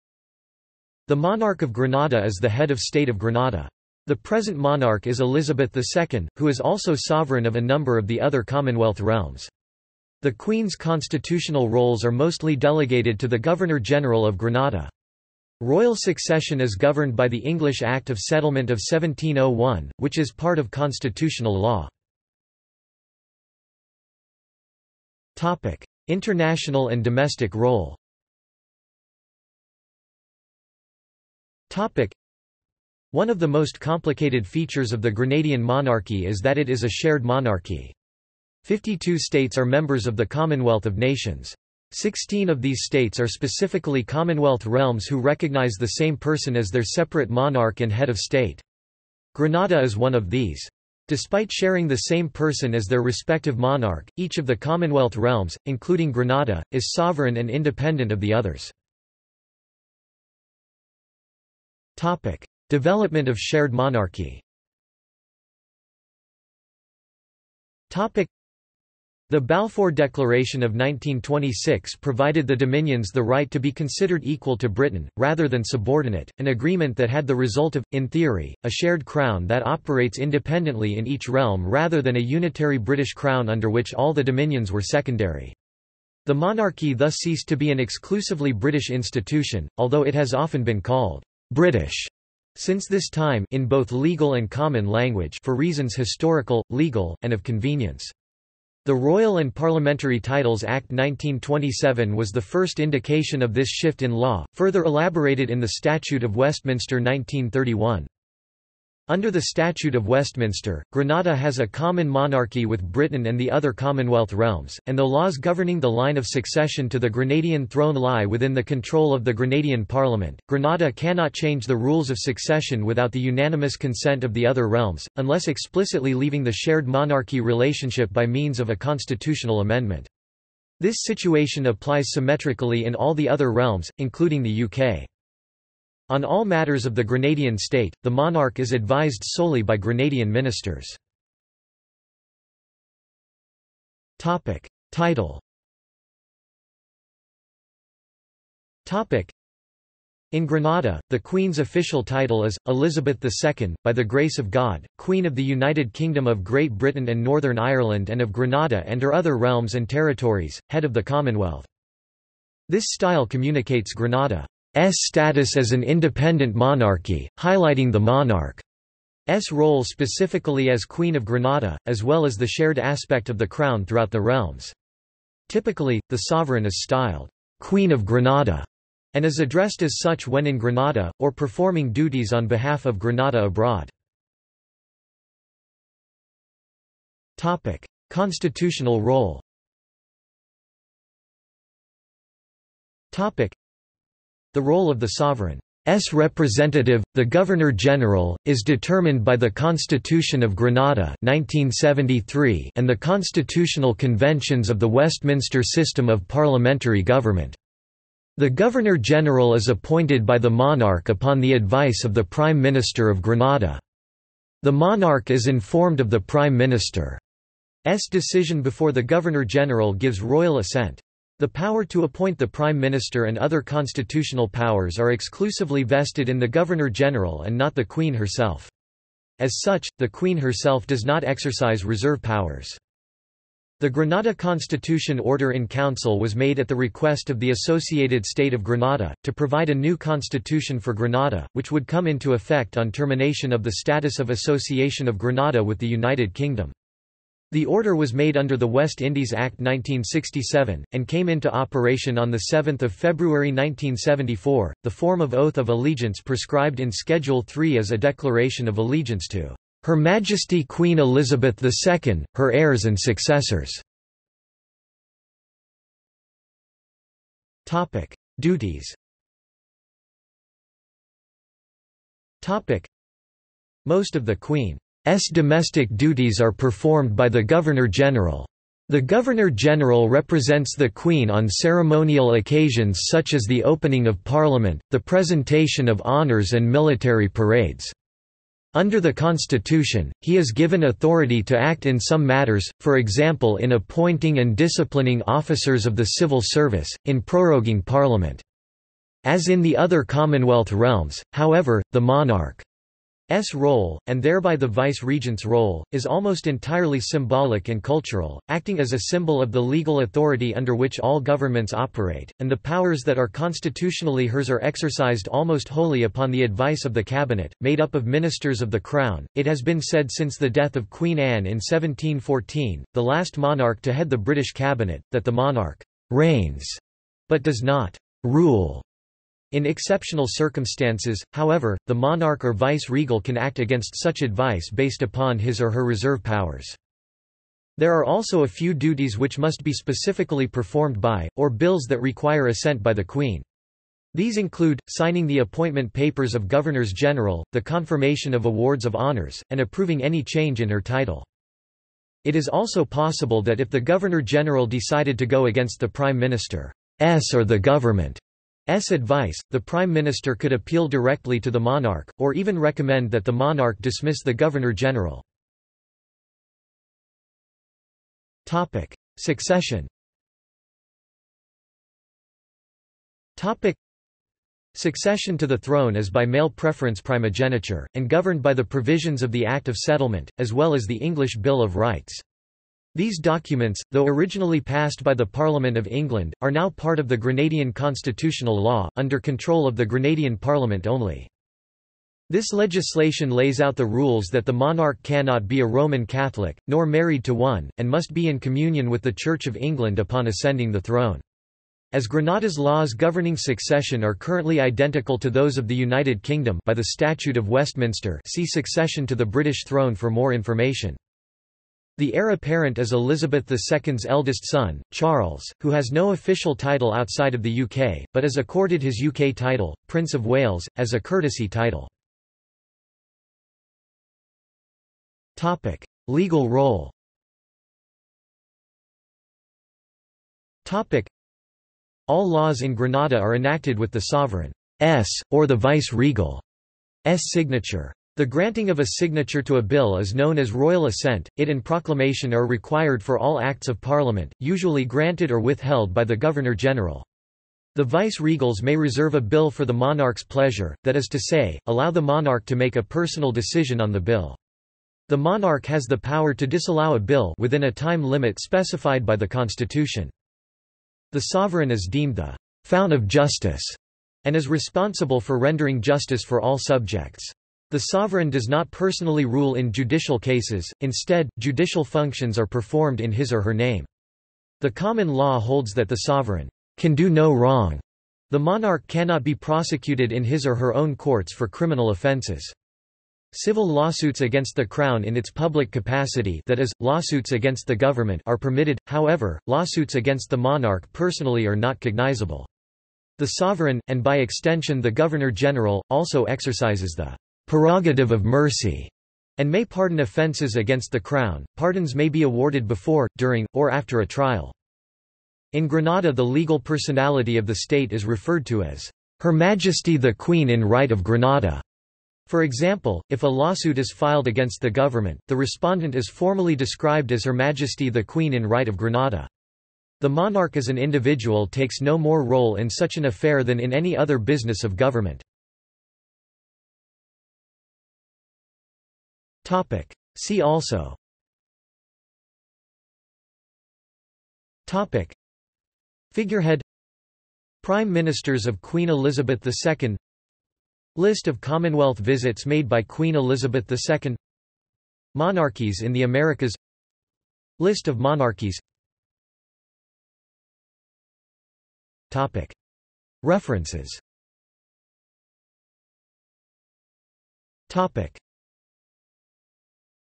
Monarchy of Grenada | Wikipedia audio article
This is an audio version of the Wikipedia Article:
Monarchy of Grenada

SUMMARY
The monarch of Grenada is the head of state of Grenada. The present monarch is Elizabeth II, who is also Sovereign of a number of the other Commonwealth realms. The Queen's constitutional roles are mostly delegated to the Governor-General of Grenada. Royal succession is governed by the English Act of Settlement of 1701, which is part of constitutional law.